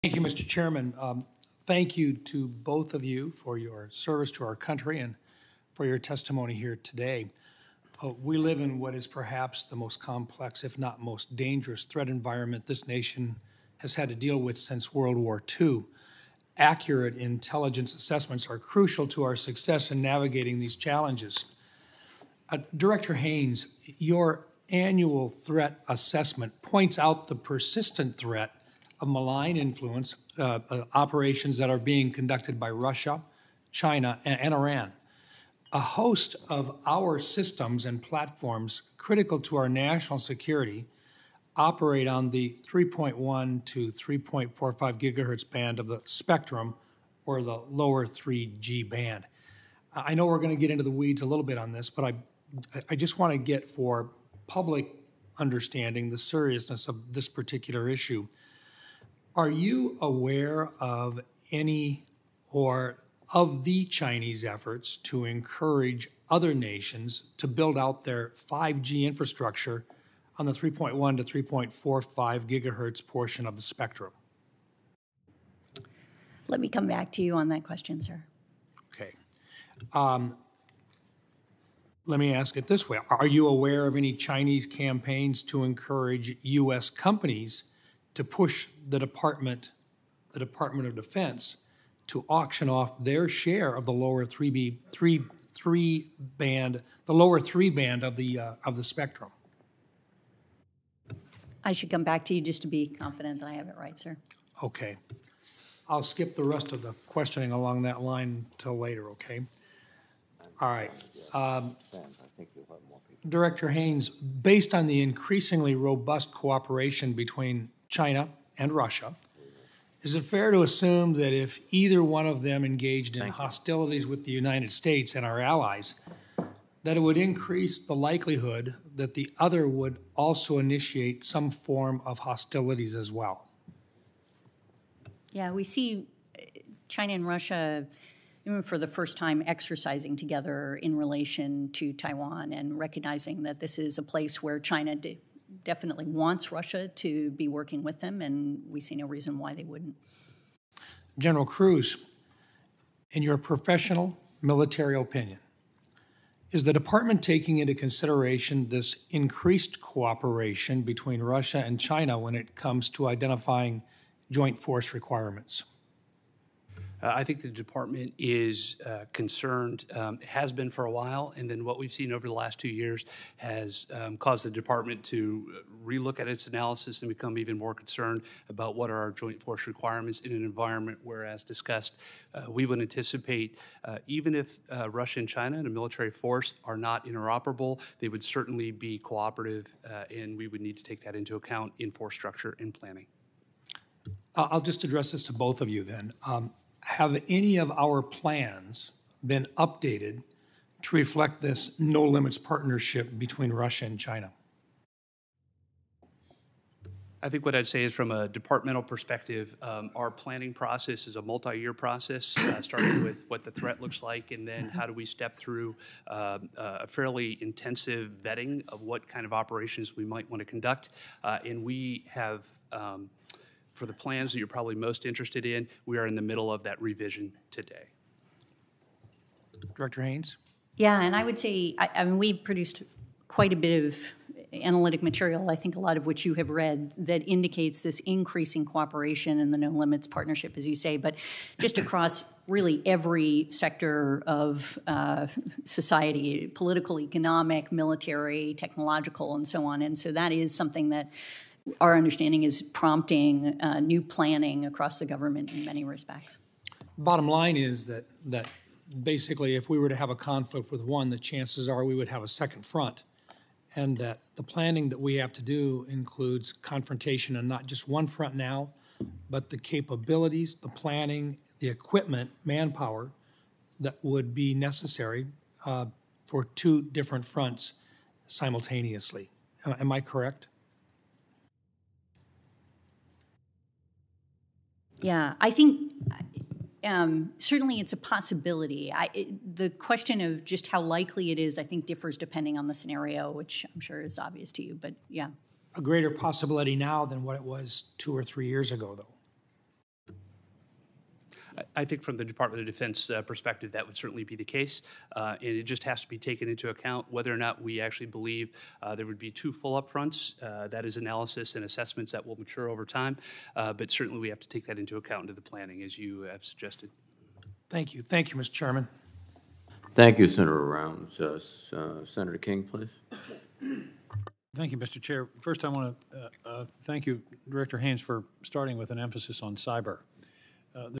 Mike Rounds Presses Officials On US Plans To Combat ‘No-Limits Partnership’ Between Russia & China
During a Senate Armed Services Committee hearing last week, Sen. Mike Rounds (R-SD) spoke to Director of National Intelligence Avril Haines about threats to the US posted by Russia & China.